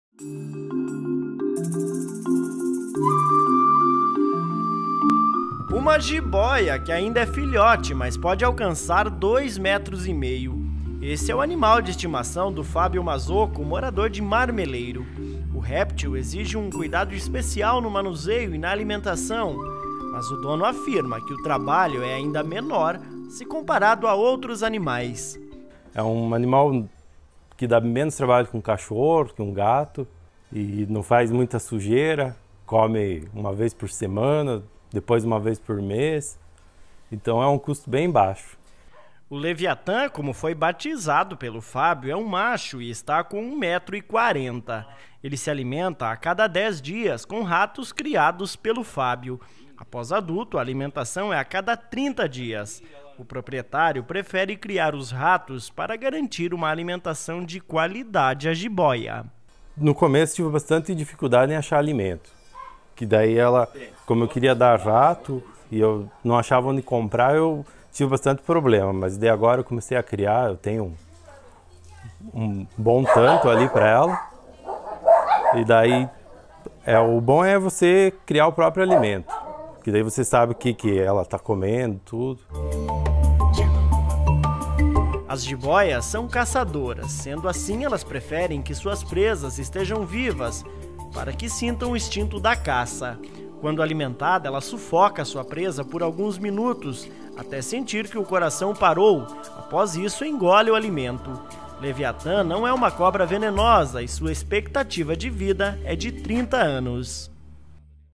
Marmeleirense tem jiboia de estimação
O réptil ainda é filhote, mas pode chegar a dois metros e meio.